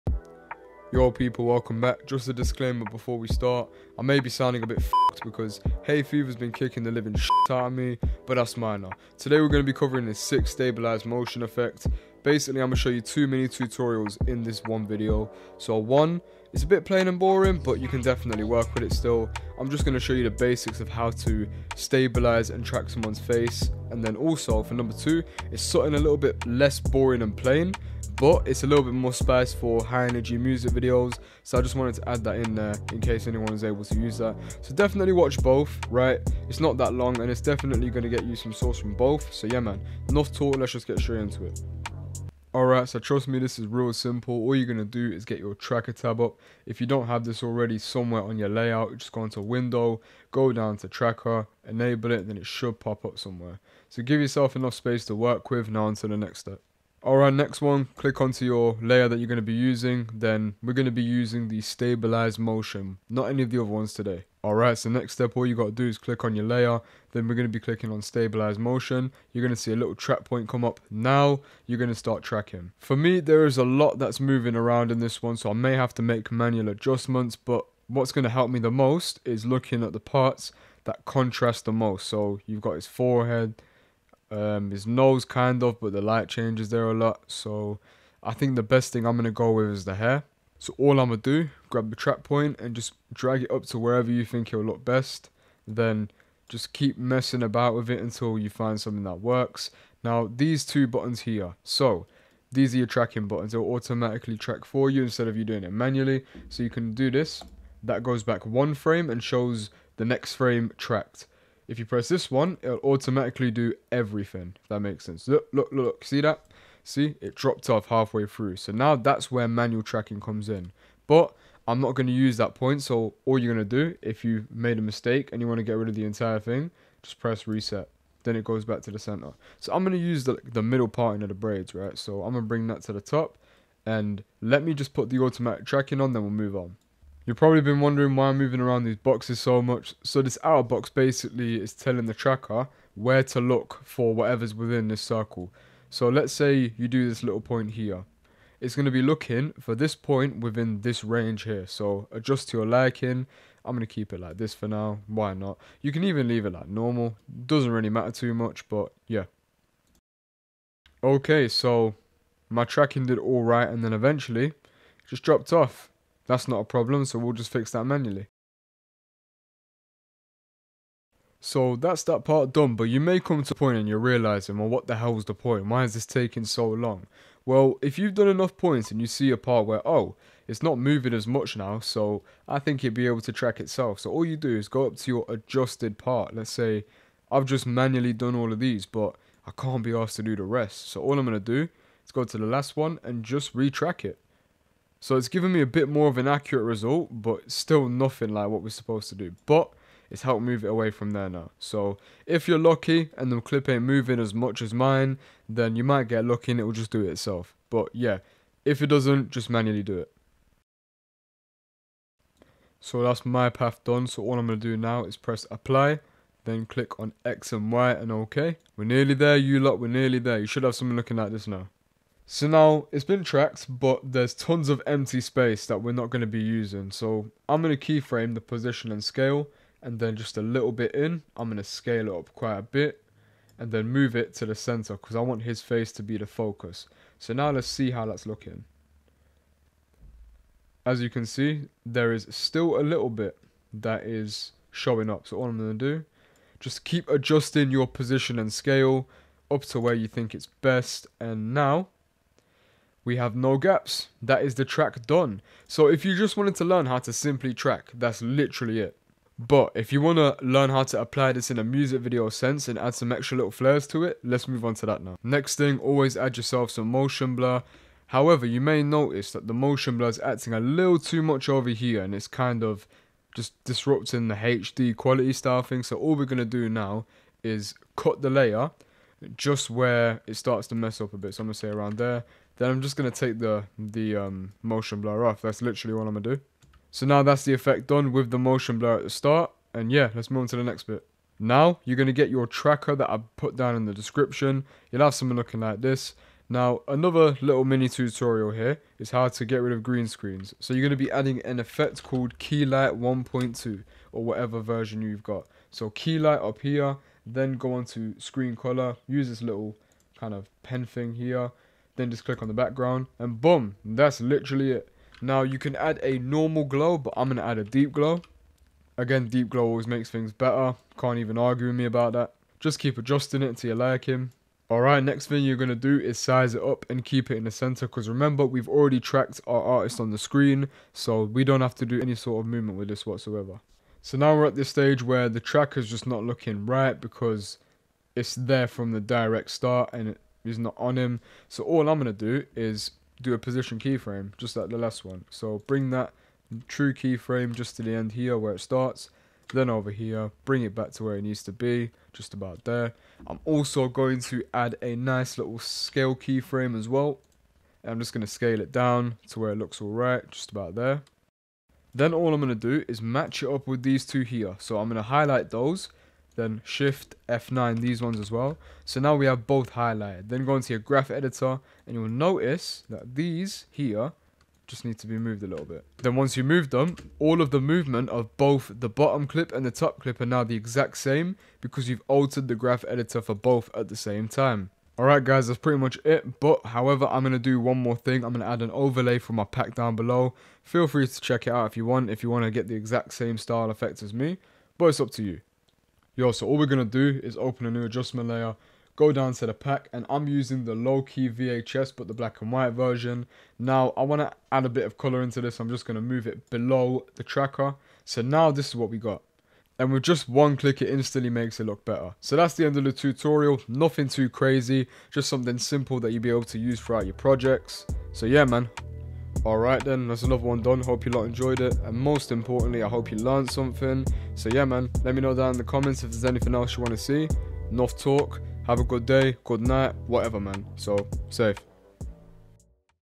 SMOOTH Stabilized Tracking Effect | After Effects Tutorial
Learn how to create a clean and smooth stabilized tracking effect in After Effects! 🎯 Perfect for music videos, creative edits, and cinematic shots, this tutorial will show you how to lock onto a subject while the background moves dynamically. No plugins required—just built-in After Effects tools to help you achieve professional results.